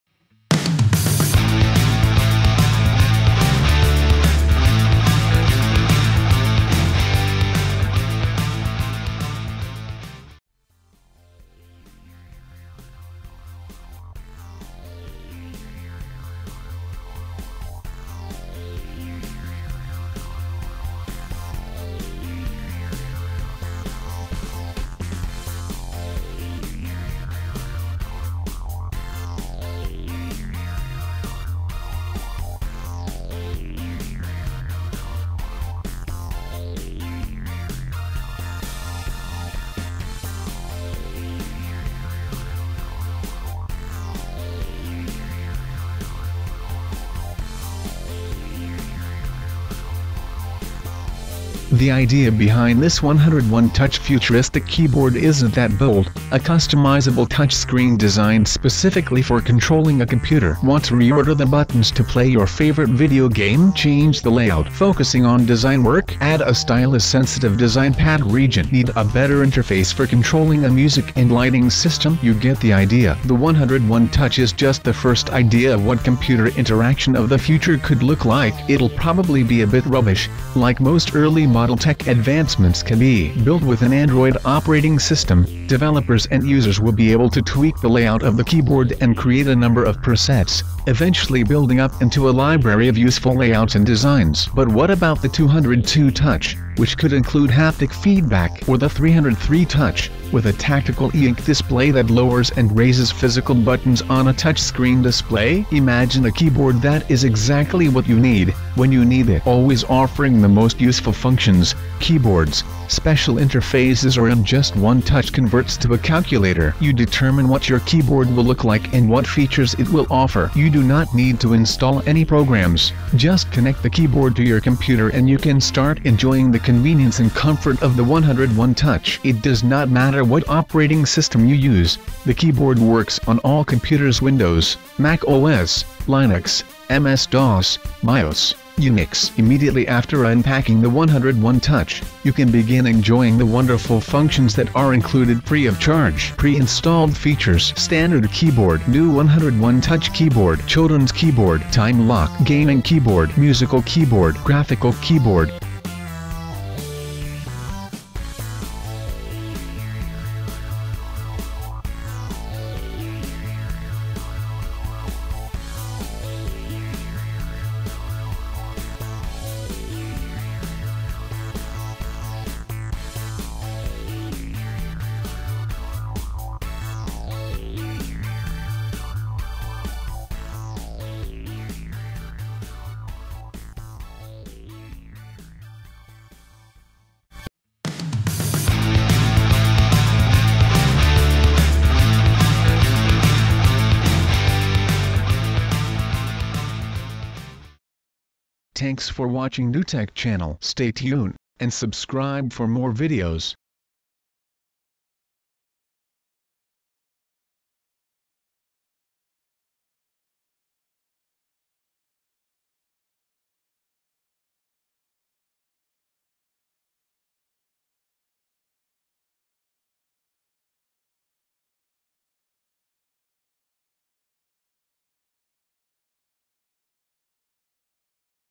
101 Touch Keyboard offer a next generation keyboard to users
The idea behind this 101touch futuristic keyboard isn't that bold: a customisable touchscreen designed specifically for controlling a computer. Want to reorder the buttons to play your favourite video game? Change the layout. Focusing on design work? Add a stylus-sensitive design pad region. Need a better interface for controlling a music and lighting system? You get the idea.

The 101touch is just the first idea of what computer interaction of the future could look like. It'll probably be a bit rubbish, like most early model tech advancements can be. Built with an Android operating system, developers and users will be able to tweak the layout of the keyboard and create a number of presets - eventually building up into a library of useful layouts and designs. But what about the 202touch, which could include haptic feedback? Or the 303touch, with a tacticle eInk display that lowers and raises physical buttons on a touchscreen display?

Imagine a keyboard that is exactly what you need, when you need it. Always offering the most useful functions, keyboards, special interfaces or in just one touch converts to a calculator. You determine what your keyboard will look like and what features it will offer. You do not need to install any programs, just connect the keyboard to your computer and you can start enjoying the convenience and comfort of the 101touch. It does not matter what operating system you use, the keyboard works on all computers (Windows, Mac OS, Linux, MS Dos, Bios, Unix... ). Immediately after unpacking the 101touch, you can begin enjoying the wonderful functions that are included free of charge.

Pre-installed features:

- Standard keyboard

- New 101touch keyboard

- Children's keyboard

- Time lock

- Gaming keyboard

- Musical keyboard

- Graphical keyboard

Welcome to nũTECH ! The leading consumer tech lifestyle channel and the ultimate YouTube destination for the latest tech news!

Now on tumblr !

Extra information